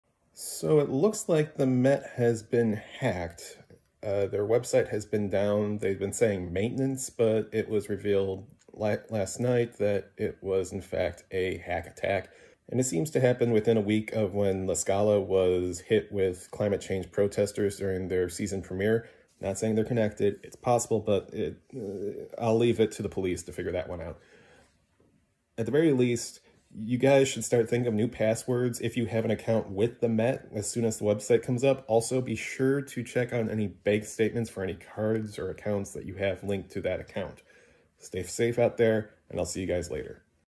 Met Opera hit with Cyberattack
After days of being down with “maintenance,” the Metropolitan Opera has confirmed that their website has been hit with a cyberattack.